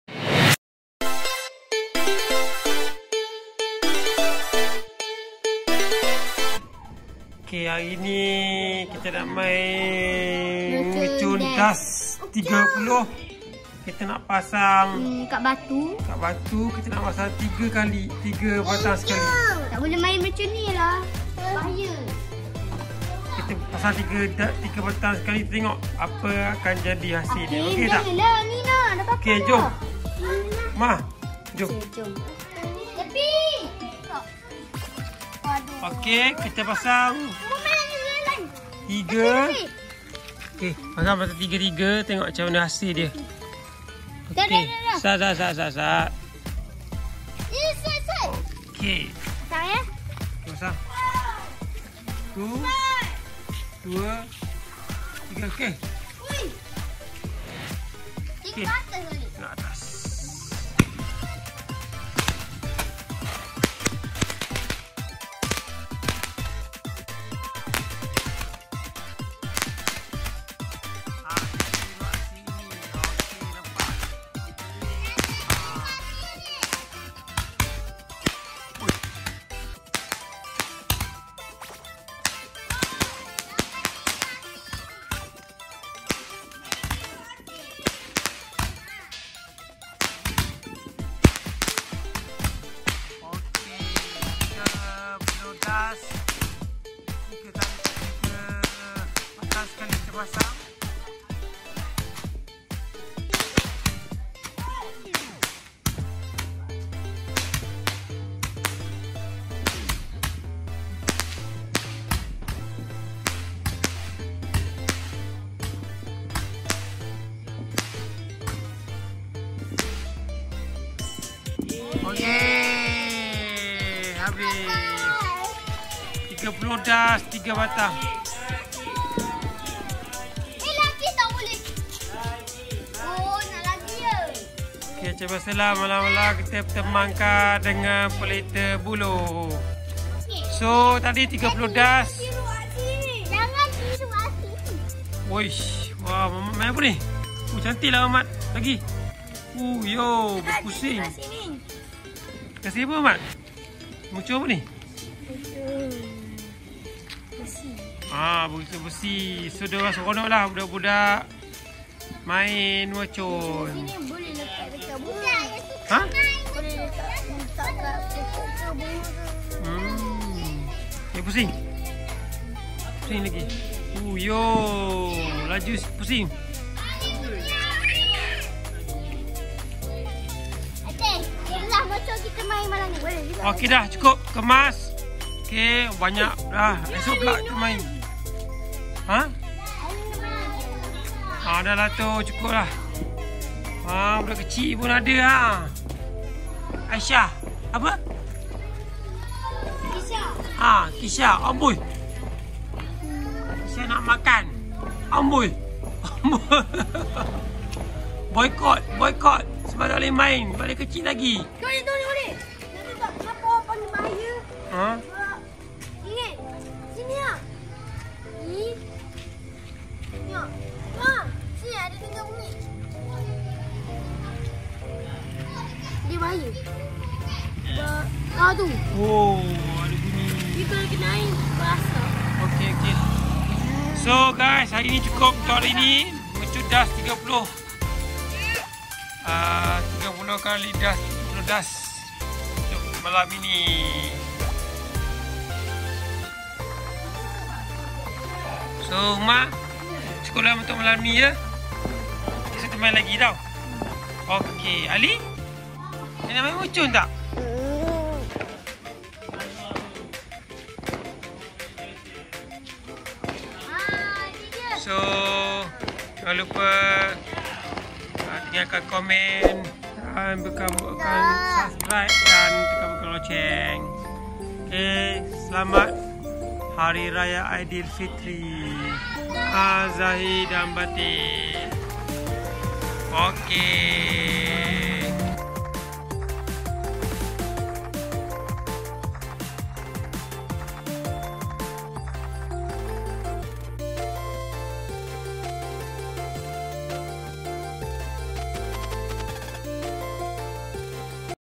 Unboxing Mercun 30 DAS PADU 2022 by mek ira
Review mercun 30 das di kg tahun 2022 bersama ank2 dan anak2 buah. Kita review dgn bakar sekali 3 batang. JGN TIRU AKSI INI.   OK Created by InShot